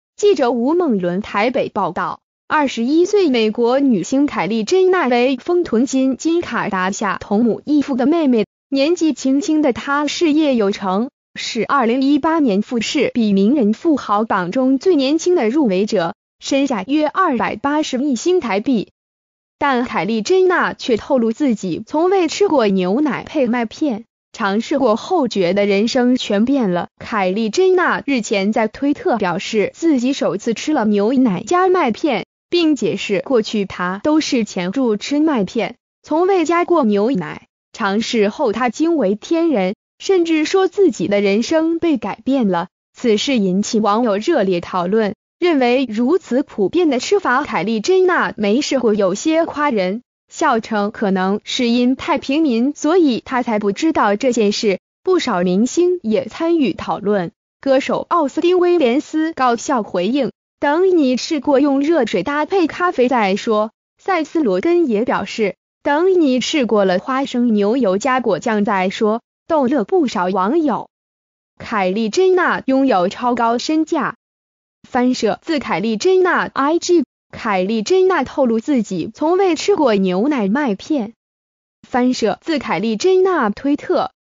身價280億女星下凡 首次吃牛奶麥片大驚「人生改變了」
〔記者吳孟倫／台北報導〕21歲美國女星凱莉珍娜為「豐臀金」金卡達夏同母異父的妹妹，年紀輕輕的她事業有成，是2018年富士比名人富豪榜中最年輕的入圍者，身價約280億新台幣。但凱莉珍娜卻透露自己從未吃過牛奶配麥片，嘗試過後覺得人生全變了。

發表評論評論如下。